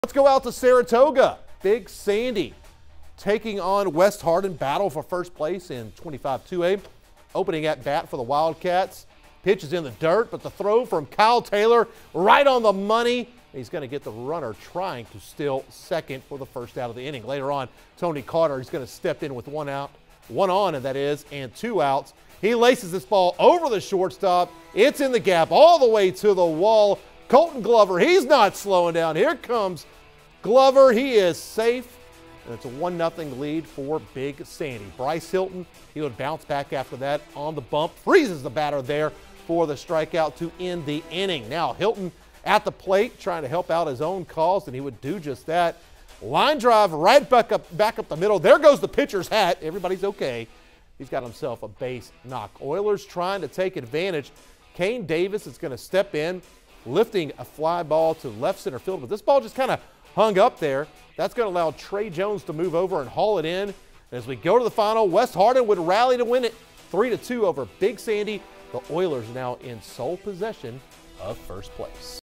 West Hardin slips past Big Sandy to take over district lead
Oilers stay perfect in 25-2A play